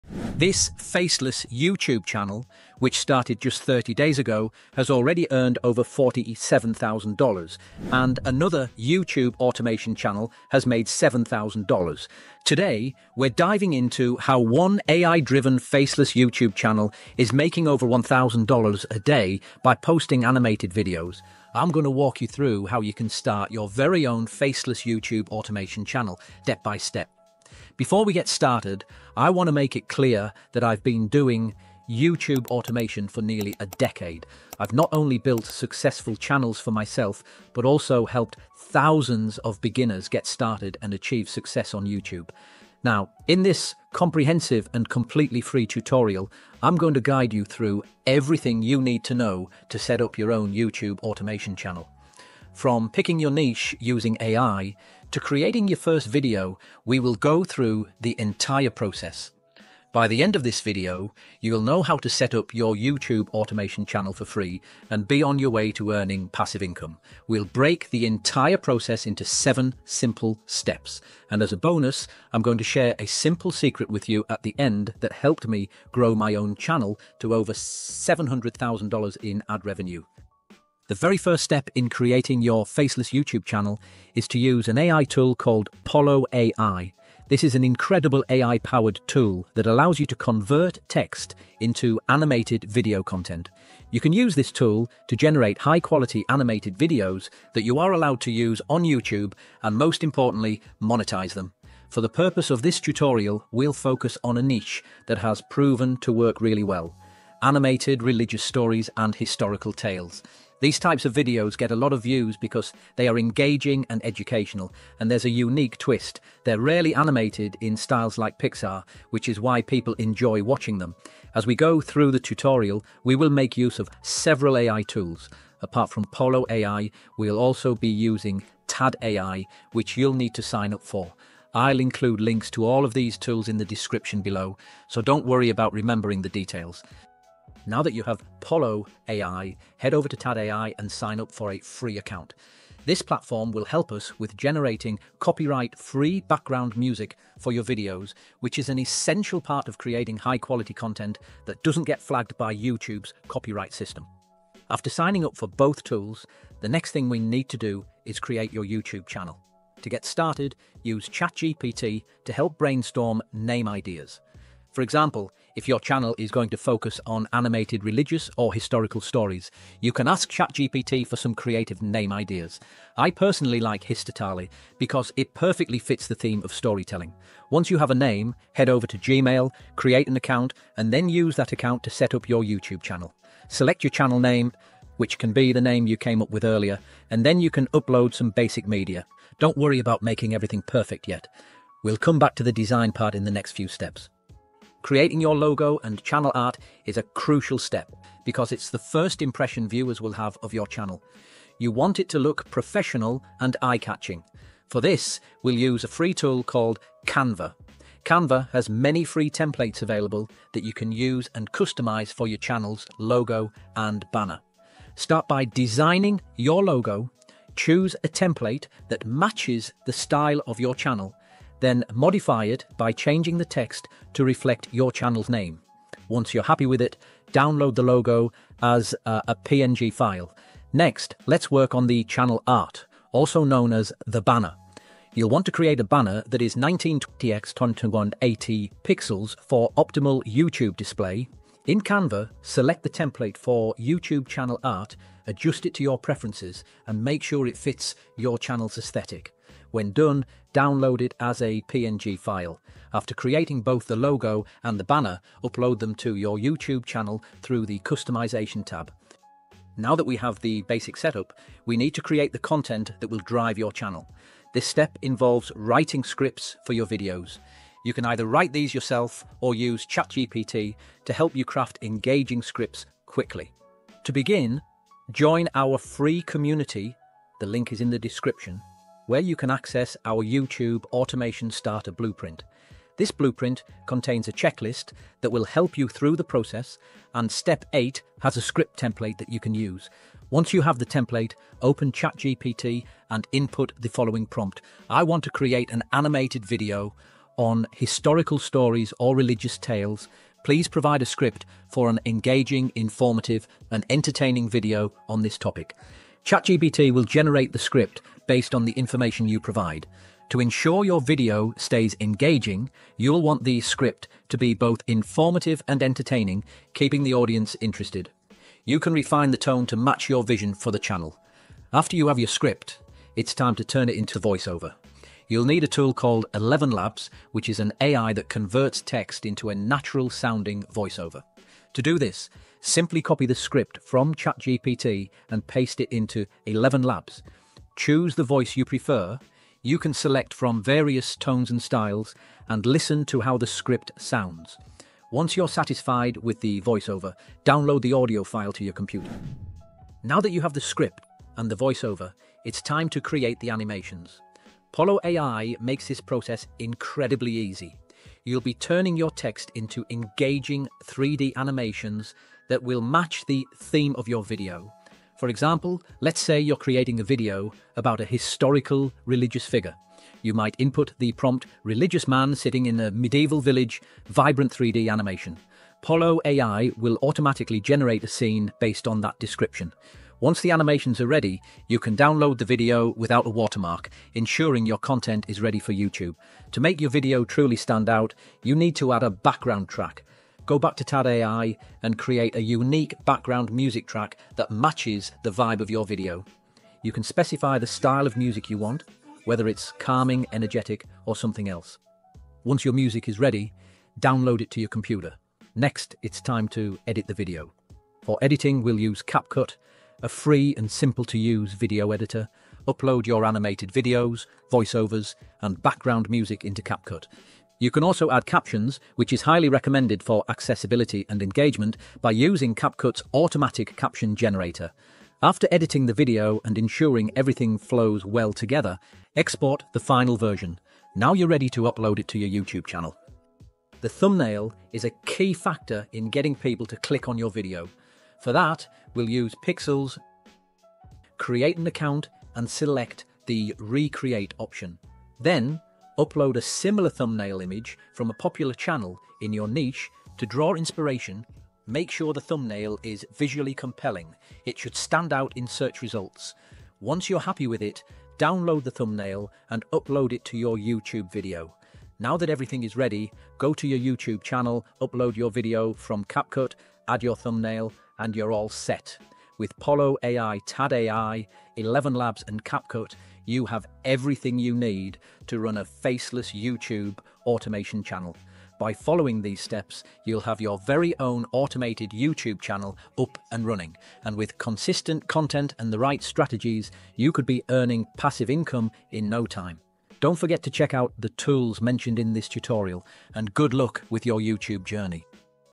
Pollo AI Video Generator Tutorial | Viral Videos FREE & UNLIMITED
🔹 ✅ Pikzels The NEWEST and BEST Thumbnail Generator - Try for 30 days

🚀 How to Start a Faceless YouTube Channel and Earn $1,000+ a Day with AI! 🚀

Want to make passive income on YouTube with no face on camera? AI is the key! In this step-by-step guide, I’ll show you how to start a faceless YouTube automation channel using powerful AI tools, allowing you to create high-quality animated videos that attract views and make money—just like the channels already earning over $47,000 in their first month!

🔥 What You’ll Learn in This Video:

How to create faceless YouTube channels using AI and automation tools.

Step-by-step guide on choosing the best niche, including animated religious stories and historical tales.

How to create animated videos with Polo AI, generate copyright-free music with Tad AI, and create realistic voiceovers using 11Labs.

Tips on setting up your channel, including choosing a name, designing logos, and creating engaging thumbnails with Pixels.

Editing tips with free tools like CapCut to polish your video and make it ready for YouTube.

💡 AI Tools Used in This Tutorial:

Polo AI – Convert text to animated video content.

11Labs – Generate natural-sounding voiceovers.

CapCut – Edit your videos with ease.

Pixels – Create clickable, attention-grabbing thumbnails.

By following this free tutorial, you’ll be able to create a passive income stream with YouTube automation, producing videos quickly and easily using just a few AI tools.

🎯 Bonus Tip: Stick around until the end for a secret strategy that helped me earn over $700,000 in ad revenue from YouTube!